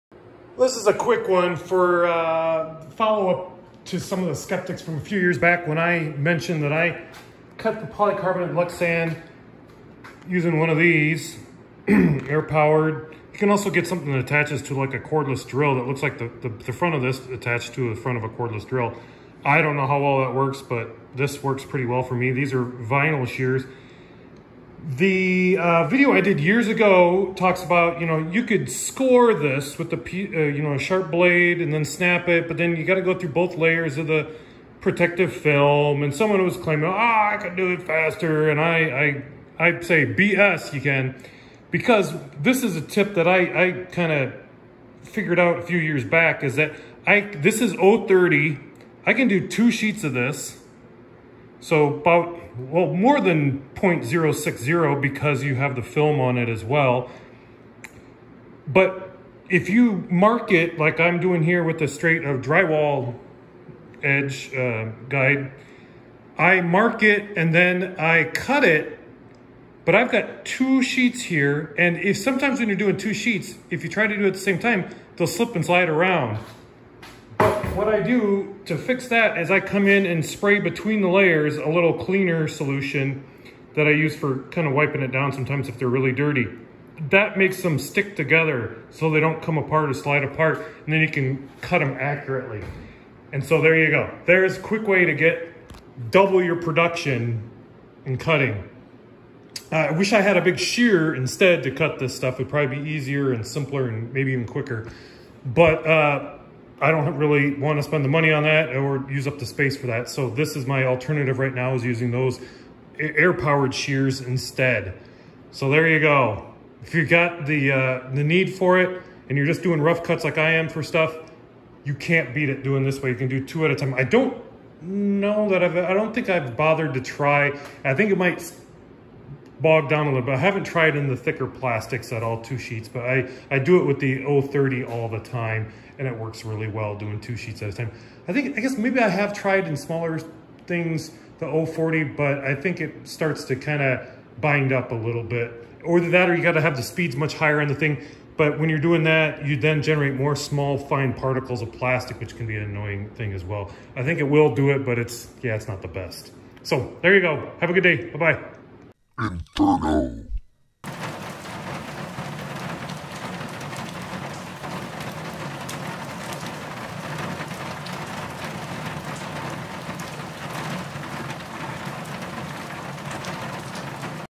Cut 2 Thin Sheets of Lexan (poly-carbonate) Plastic at the Same Time
It can be done easy with hand held shears.